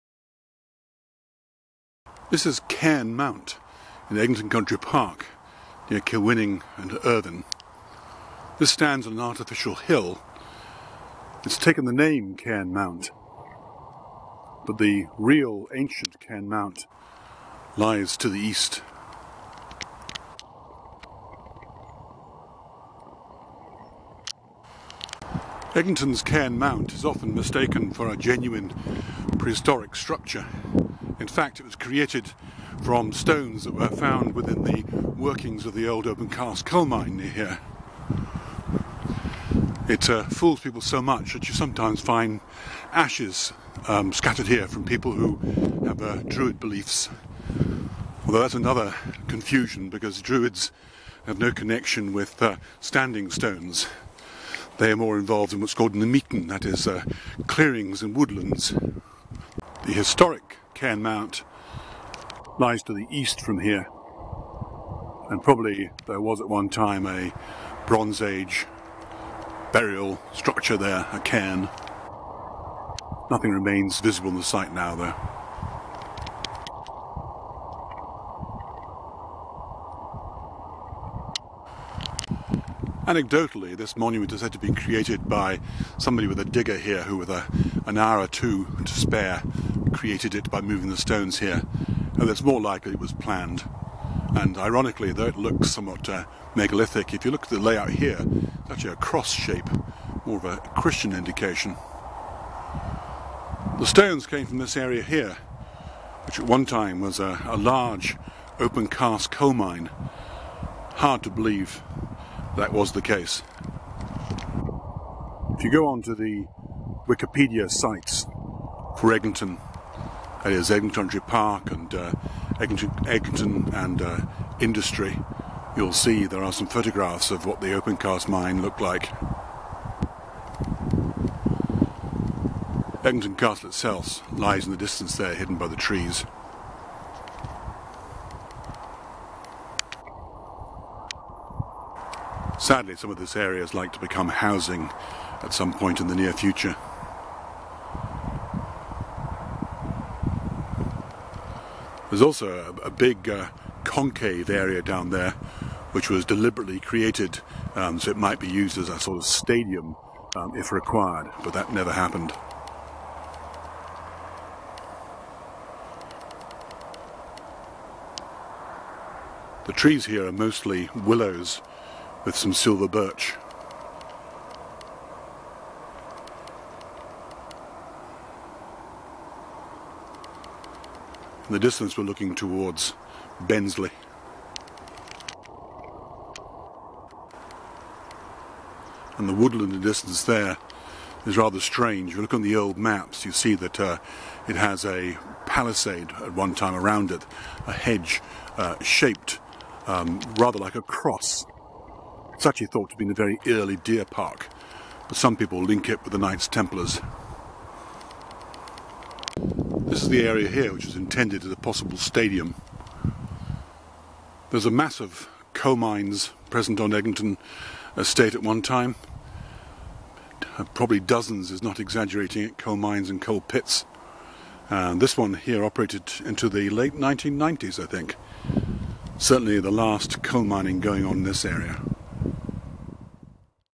The Cairn Mount Stones & the Old Sourlie Coal Mine
The story of the new megalith built at Cairn Mount in Eglinton Country Park and the old Sourlie Open Cast Mine - the last mine in Irvine after 500 years of coal mining.